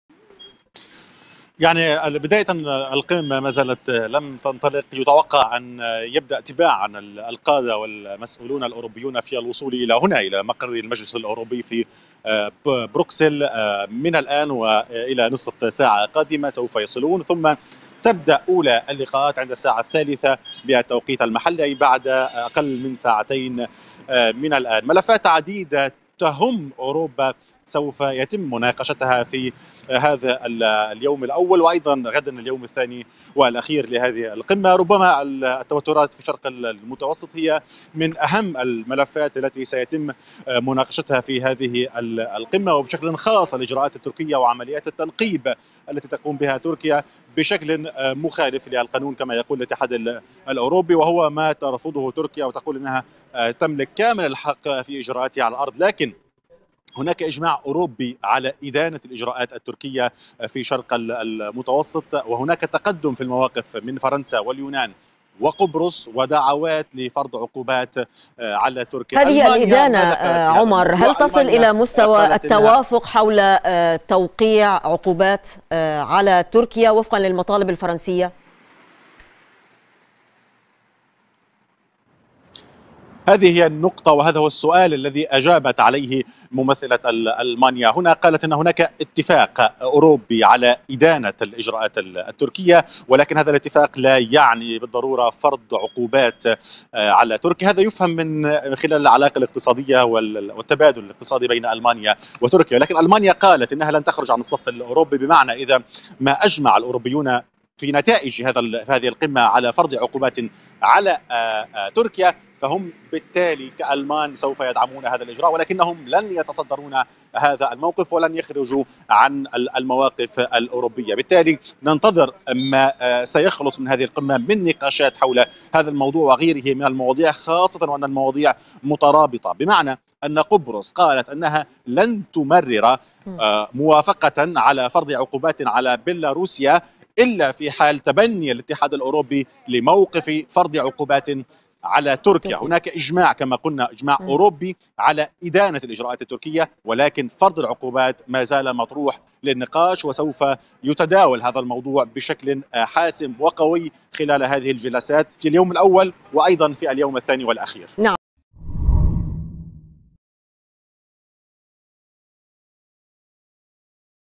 ملفات عديدة سيتم مناقشتها في القمة الأوروبية ببروكسل.. فما هي؟
يبحث رؤساء دول  وحكومات الاتحاد الأوروبي اليوم في بروكسل التوتر في شرق المتوسط والعلاقة بين تركيا ودول الاتحاد.

يأتي ذلك بينما طالب وزير الدولة الفرنسي للشئون الأوروبية كليمنت بيون، الاتحاد الأوروبي, باتخاذ مواقف حازمة تجاه أنقرة.
. المزيد من التفاصيل يتحدث مراسل الغد من بروكسل عمر بشير